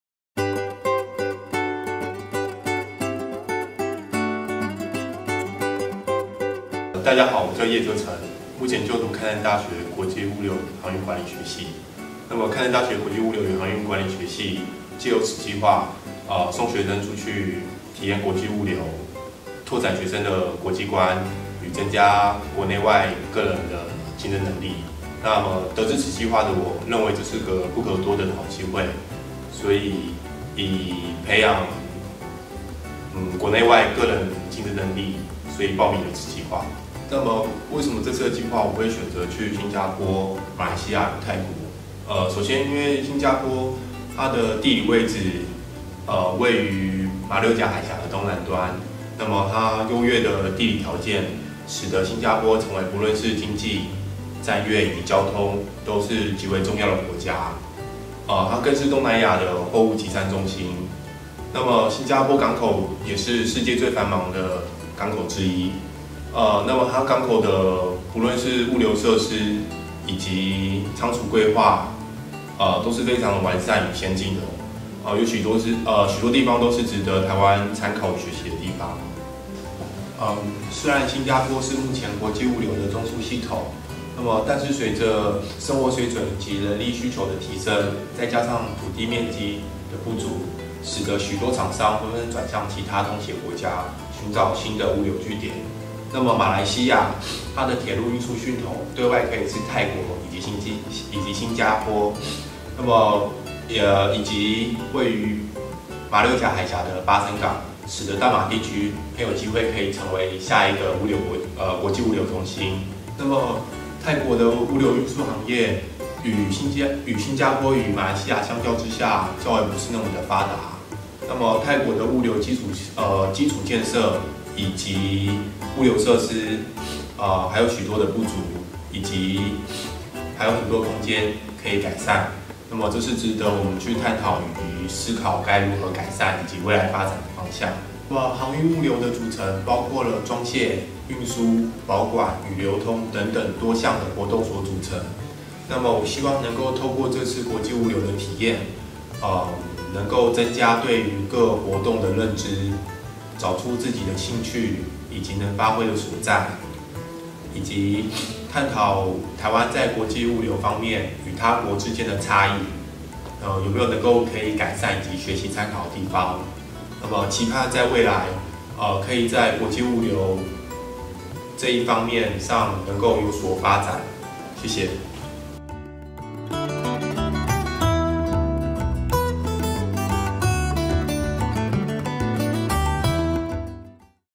開南物流菁英計畫 新馬泰壯行 葉哲辰
開南物流菁英計畫選送學生出國，實地體驗國際物流作業，培養國際級人才